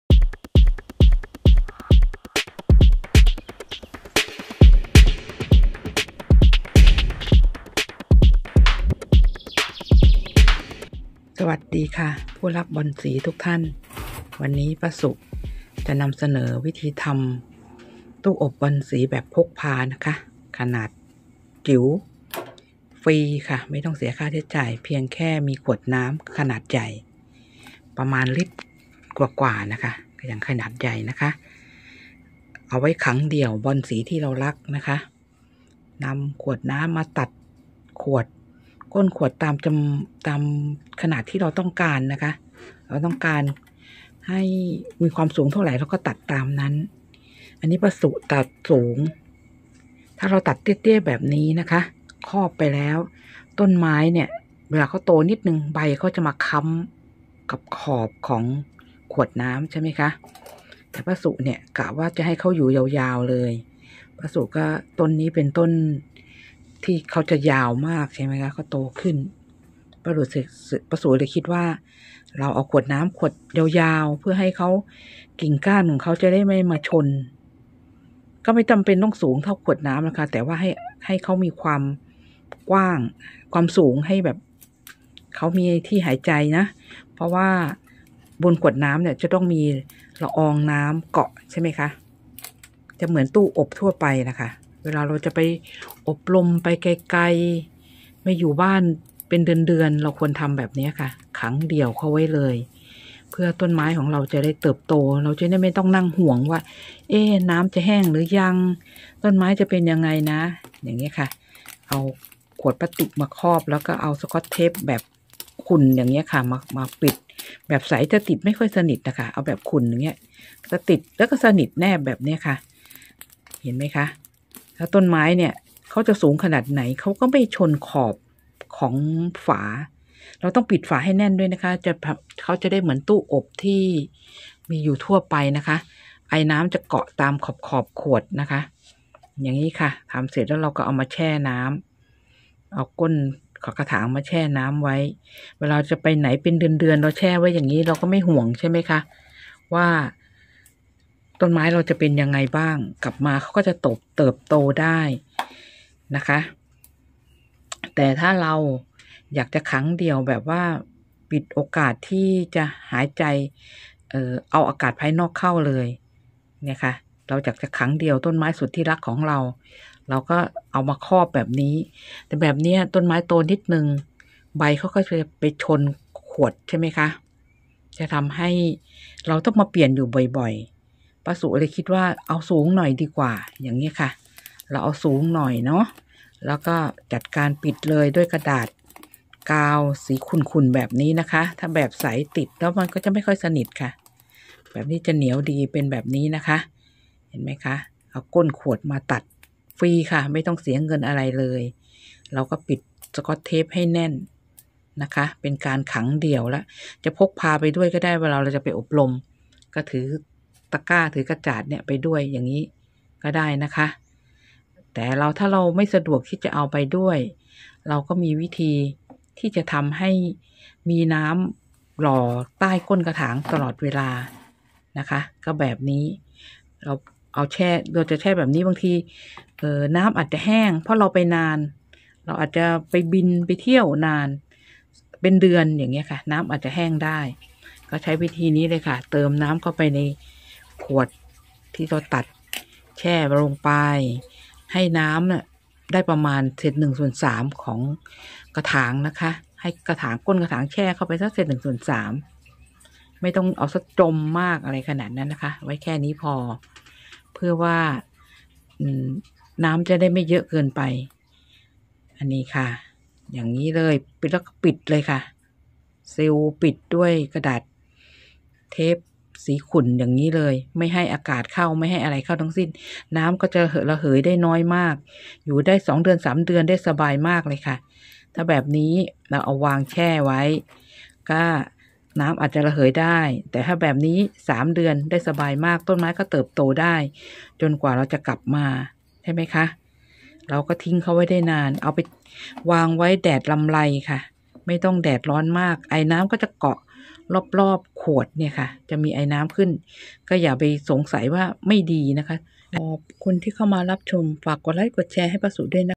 ฟรี! ตู้อบบอนสีแบบพกพา ทำง่ายได้ผลจริง บอนสีโตไว ขังเดี่ยวบอนสีที่เรารัก
@SU-indy ขอนำเสนอวิธีทำตู้อบบอนสี เป็นการขังเดี่ยวบอนสีที่เรารัก ให้เติบโตไวๆ เป็นวิธีทำที่ง่ายๆได้ผลชัวร์ ฟรีไม่ต้องเสียเงิน ไปซื้ออุปกรณ์แพงๆมาใช้ สิ่งที่ต้องเตรียมมีดังนี้
1.ขวดน้ำเปล่าใสขนาด 1.25 ลิตร
2.สก๊อตเทปสีขุ่น
3.กรรไกรตัดสก๊อตเทป
4.ต้นไม้และกระถางที่เราต้องการทำ
เมื่อเราได้อุปกรณ์ตามต้องการก็จัดการตัดขวดน้ำตามขนาดที่เราต้องการ  ครอบไปในกระถางต้นไม้ปิดด้วยสก๊อตเทปสีขุ่นให้สนิท แค่นี้ต้นไม้ของเราก็ได้รับการปกป้องแล้ว อย่าลืมที่จะปิดปากขวดน้ำให้สนิท เพื่อไม่ให้อากาศภายนอกเข้า นำขวดที่เราปิดให้สนิทแล้วไปแช่น้ำ ตากแดดรำไรต้นไม้ก็จะเติบโตสวยงาม หรือถ้าเรากลัวน้ำจะแห้งไม่ต้องเติมน้ำบ่อยๆเราก็นำน้ำใส่ก้นกระถางไว้เศษ 1 ส่วน 3 แล้วปิดสก๊อตเทปให้สนิท แค่นี้เราก็ไม่ต้องเติมน้ำใน 3 เดือนต้นไม้ก็จะเติบโต เมื่อเราไม่มีเวลาจะดูแลต้นไม้หรือต้องออกไปประชุมไกลๆเป็นเวลานานๆแล้วก็ใช้วิธีนี้ในการดูแลต้นไม้หรือจะพกพาไปด้วยก็สะดวกเพราะขวดเล็กๆสามารถพกพาไปไหนก็ได้ถือเป็นตู้อบบอนสีชนิดพกพาได้สบายมากเลยค่ะ